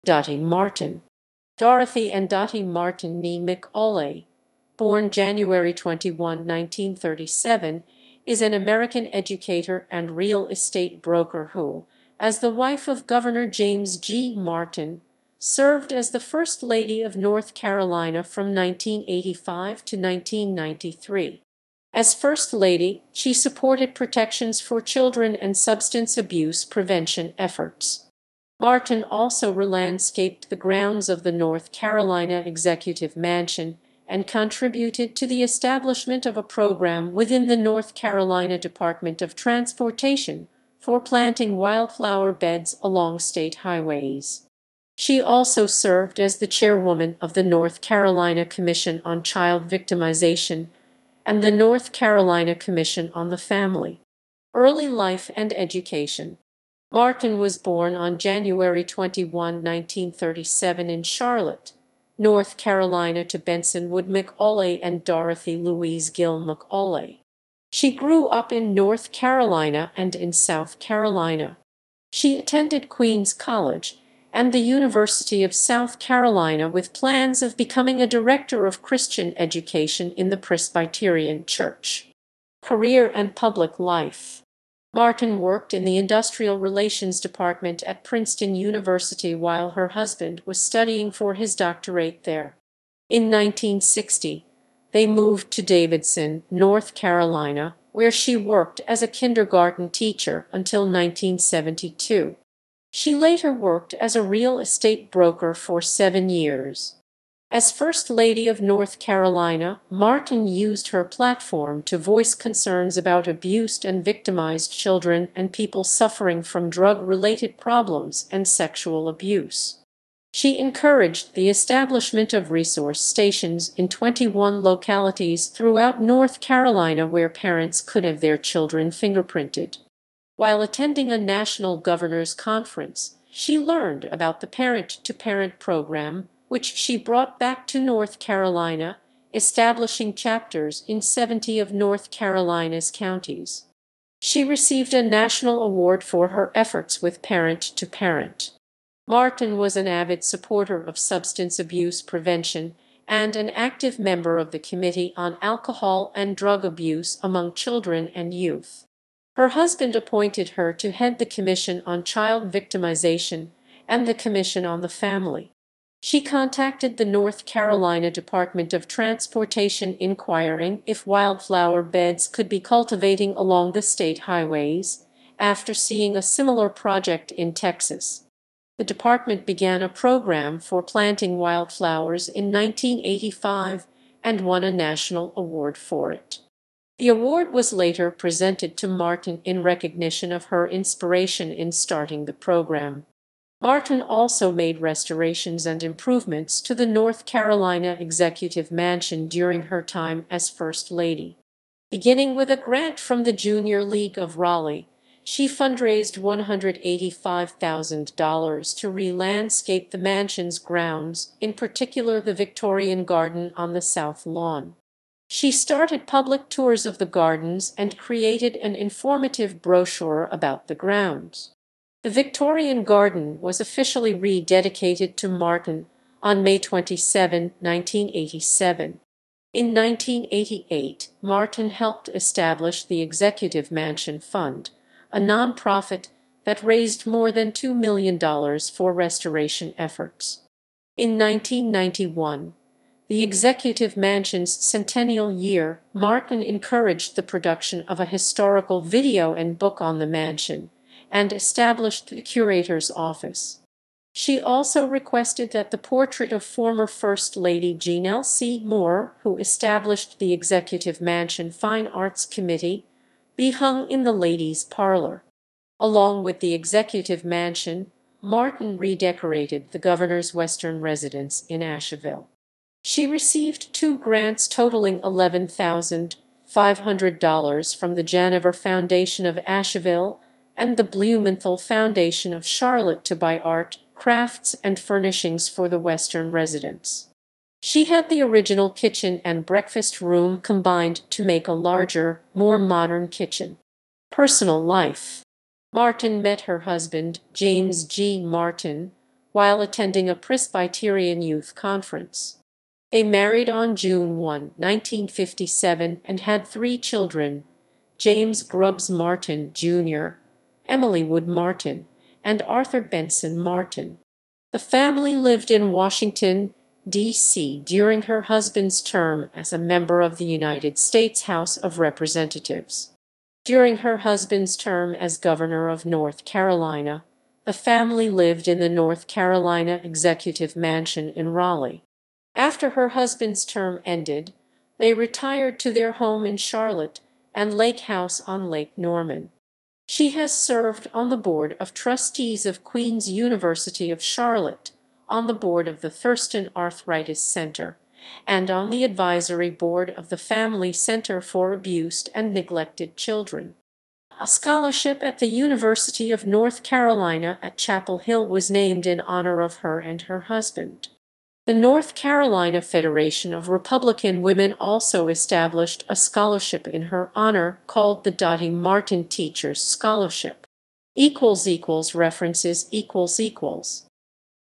Dottie Martin
Dorothy Ann "Dottie" Martin (née McAulay; born January 21, 1937) is an American educator and real estate broker who, as the wife of Governor James G. Martin, served as the First Lady of North Carolina from 1985 to 1993. As first lady, she supported protections for children and substance abuse prevention efforts. Martin also relandscaped the grounds of the North Carolina Executive Mansion and contributed to the establishment of a program within the North Carolina Department of Transportation for planting wildflower beds along state highways. She also served as the chairwoman of the North Carolina Commission on Child Victimization and the North Carolina Commission on the Family.